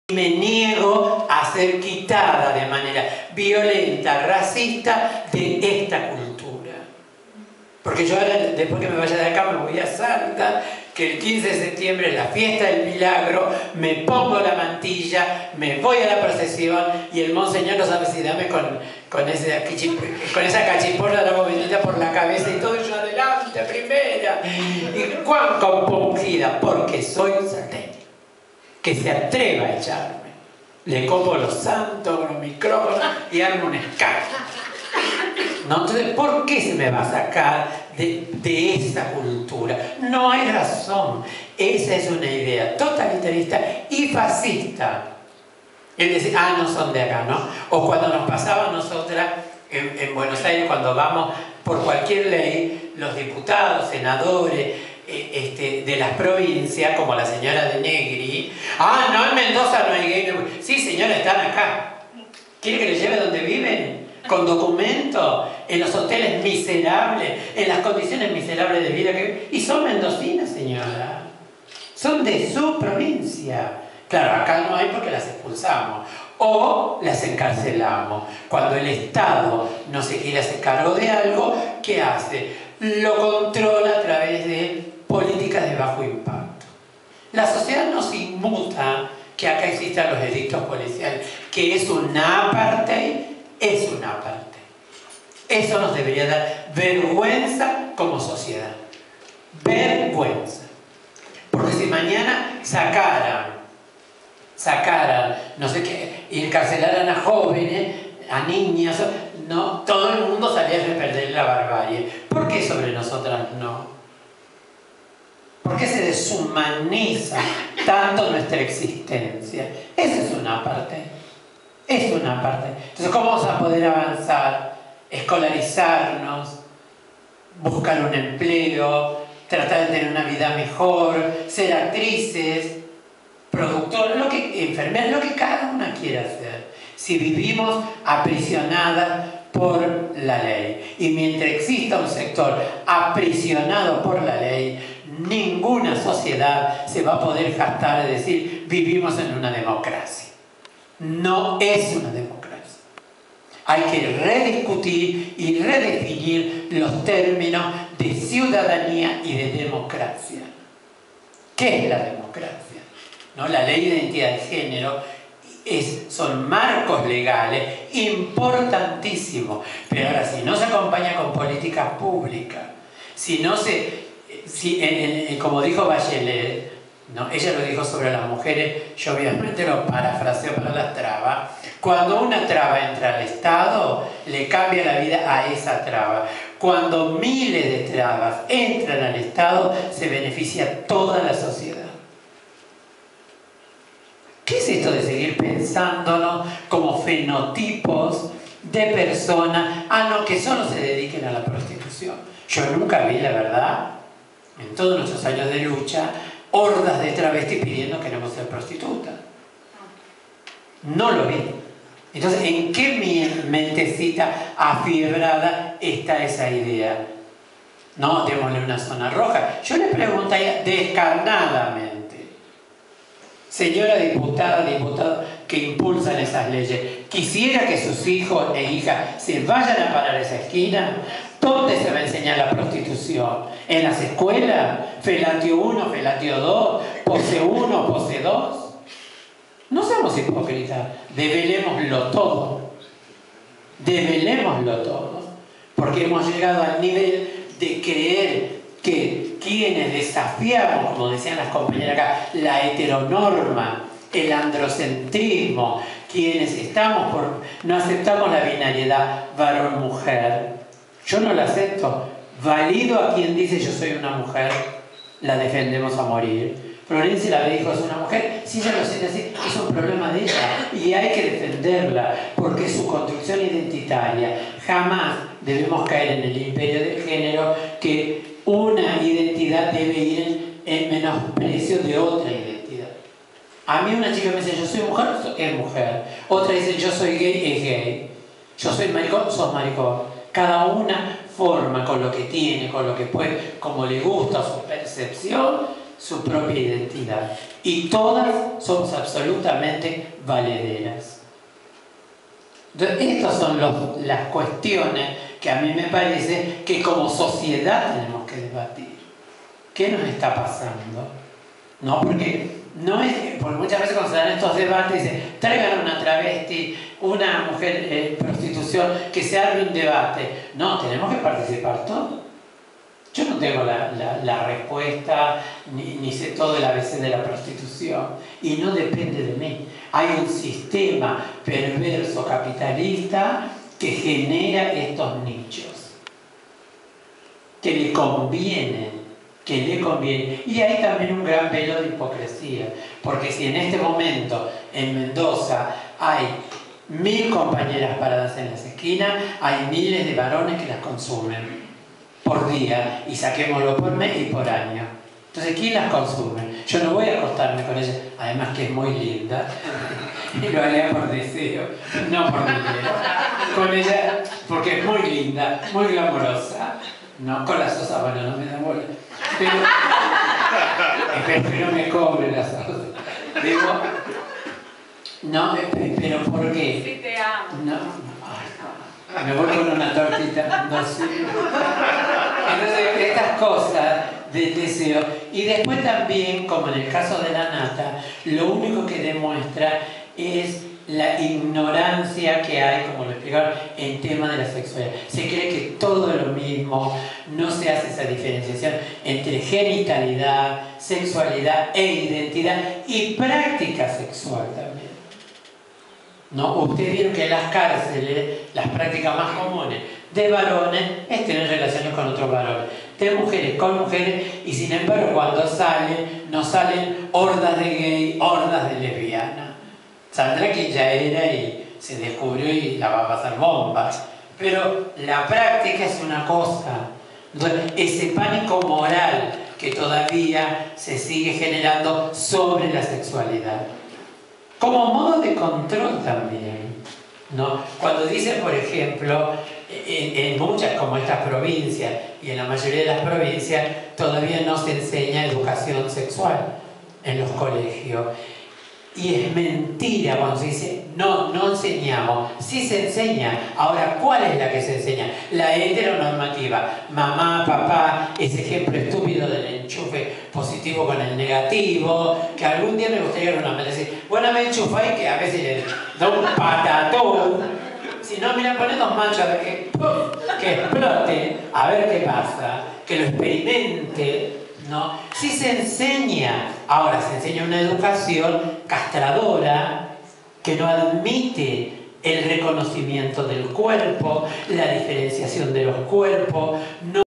Lohana Berkins en conferencia
Charla sobre "Diversidad y cine" en el festival de cine Mirada Oeste  2014, de Godoy Cruz, Mendoza. Martes 9 de septiembre. Imperdible fragmento..